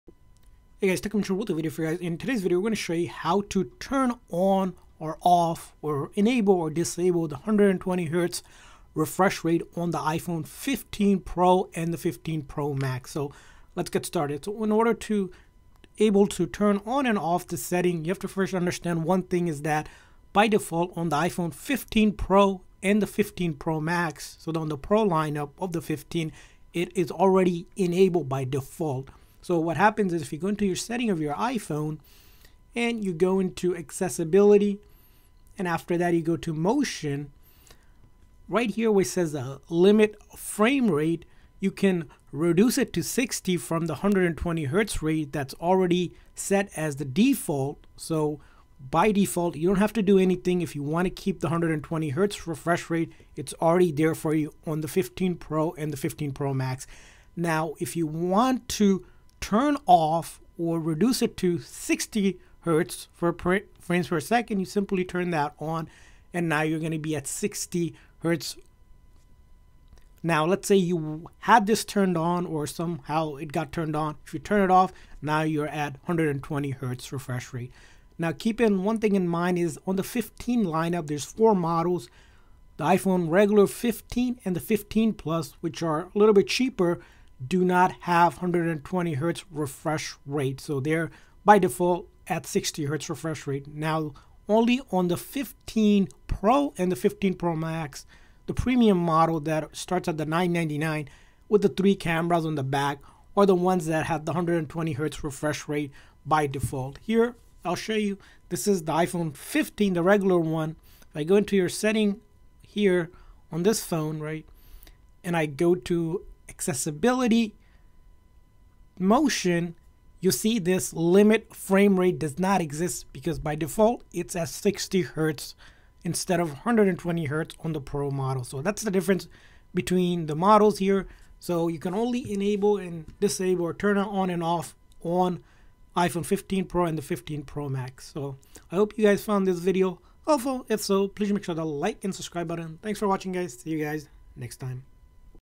iPhone 15 Pro/Pro Max: How to Turn ON/OFF 120Hz Pro Motion Refresh Rate!
iPhone 15 Pro and iPhone 15 Pro Max has 120Hz Pro Motion display refresh rate by default. In this video we show you the setting to turn off 120Hz refresh rate. Alternatively, you can turn on / enable 120Hz by turning off the same toggle setting if it's already turn on.